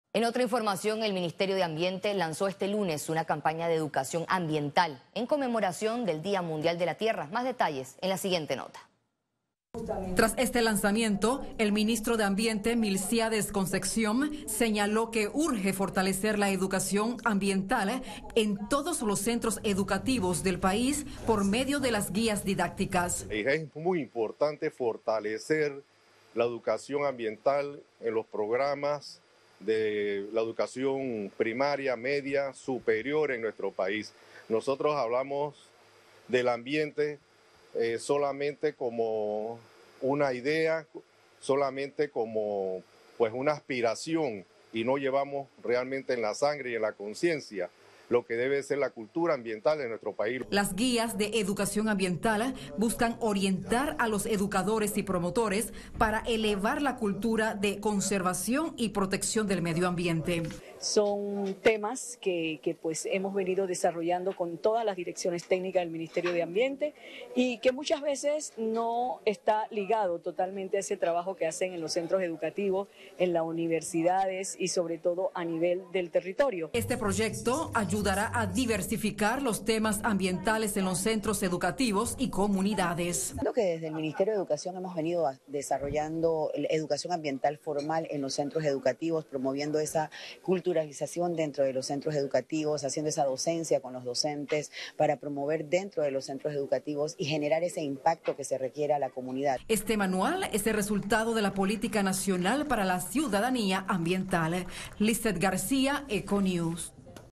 Miambiente lanza campaña nacional de educación ambiental
Fecha: 22 de Abril del 2024

Información por: Lissette García

El Ministerio de Ambiente lanzó este lunes, una campaña de educación ambiental, en conmemoración del Día Mundial de la Tierra.

Tras este lanzamiento, el Ministro de Ambiente, Milcíades Concepción, señaló que urge fortalecer la educación en todos los centros educativos del país, por medio de las guías didácticas.

“Es muy importante fortalecer la educación ambiental en los programas, la educación primaria, media, superior de nuestro país, nosotros hablamos del ambiente solamente como una idea, solamente como una aspiración y no llevamos en la sangre y en la conciencia lo que debe ser la cultura ambiental en nuestro país”, dijo Milcíades Concepción, Ministro de Ambiente.

Las guías de educación ambiental buscan orientar a los educadores y promotores, para elevar la cultura de conservación y protección del medio ambiente.

“Son temas que hemos venido desarrollando con todas las direcciones técnicas del Ministerio de Ambiente y que muchas veces no está ligado a ese trabajo que hace en los centros educativos, en la universidades y sobre todo a nivel del territorio” , explicó Melitza Tristán, directora nacional de cultura ambiental de Miambiente

Este proyecto, ayudará a diversificar los temas ambientales en los centros educativos y comunidades.

“Desde el Ministerio de Educación hemos venido desarrollando educación ambiental formal en los centros educativos promoviendo esa culturización dentro de los centros educativos haciendo esa docencia con los docentes para promover dentro de los centros educativos y promover el impacto que se requiere a la comunidad”, señaló Carmen Aparicio , directora nacional de educación ambiental del Meduca.

Este manual es el resultado de la política nacional para la ciudadanía ambiental.